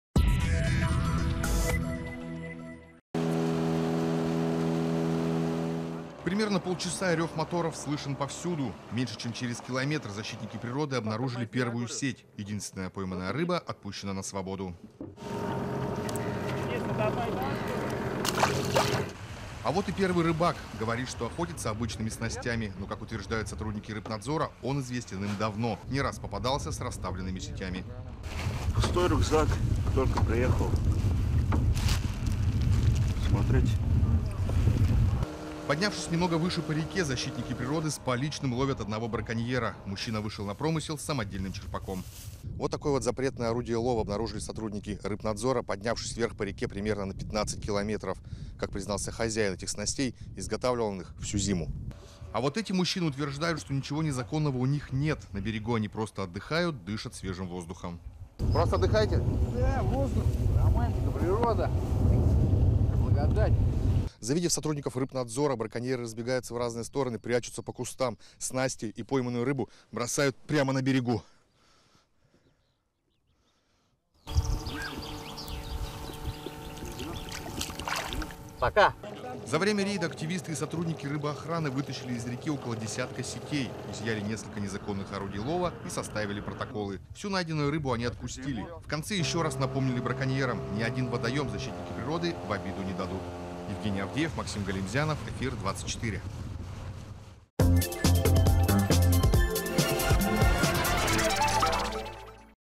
Инспекторы рыбоохраны провели экстренный рейд
Нижнекамские инспекторы рыбоохраны отправились в незапланированный экстренный рейд по реке Зай.